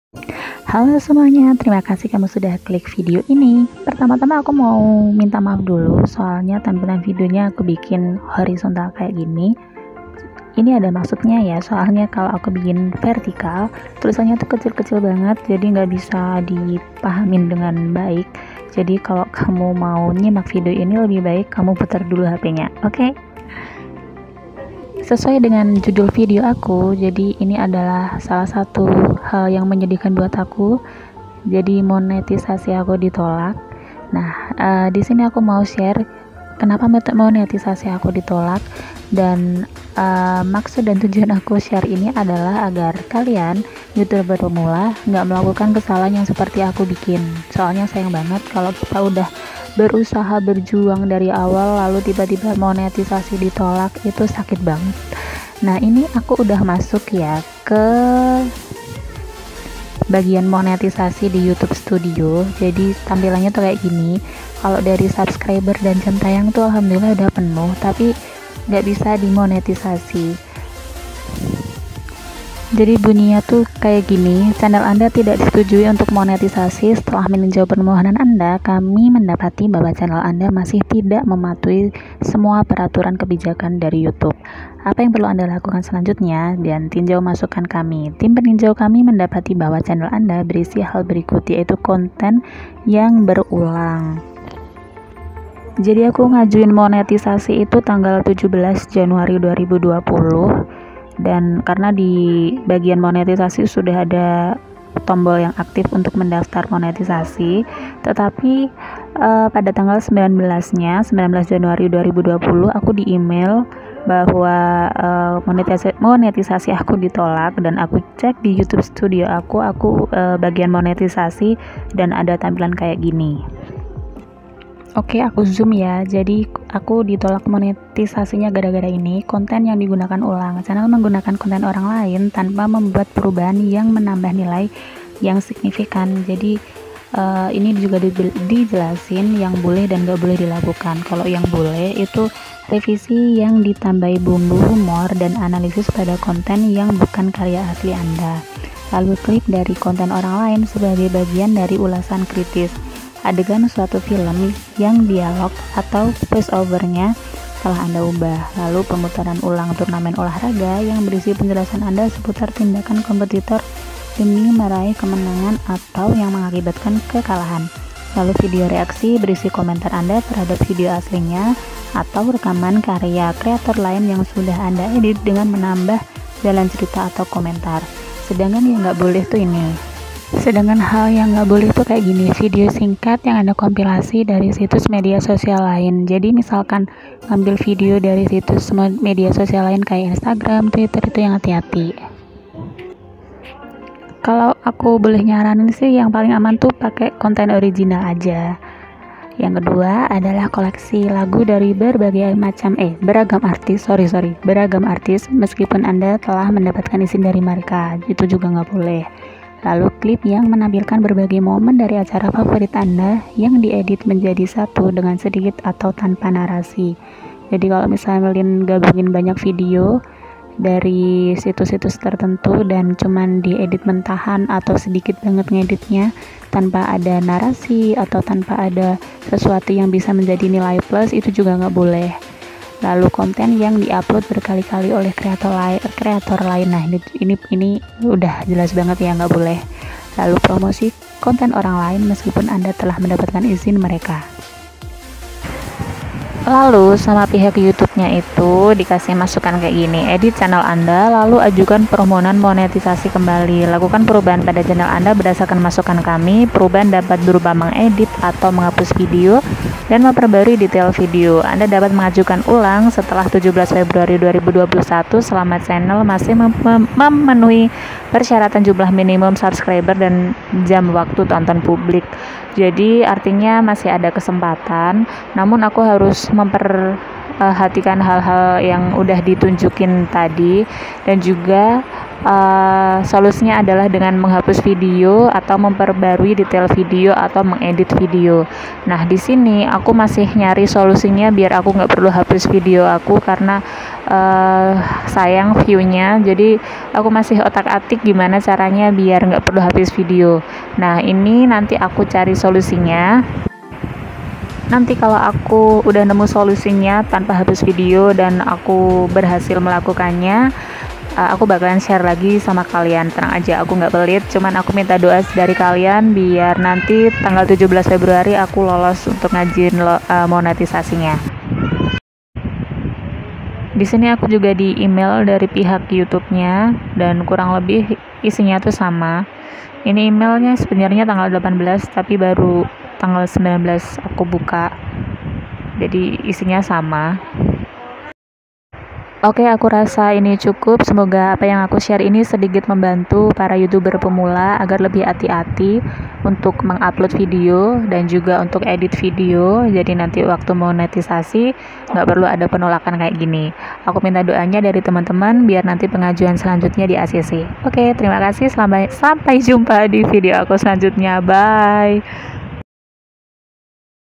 DITOLAK MONETISASI YOUTUBE KARENA INI | HATI HATI KONTEN BERULANG
Semoga pengalamanku ditolak monetisasi youtube ini bermanfaat buat semuanya terutama para youtuber pemula. Sehingga lebih berhati hati.
Have a nice day
Silahkan share bila bermanfaat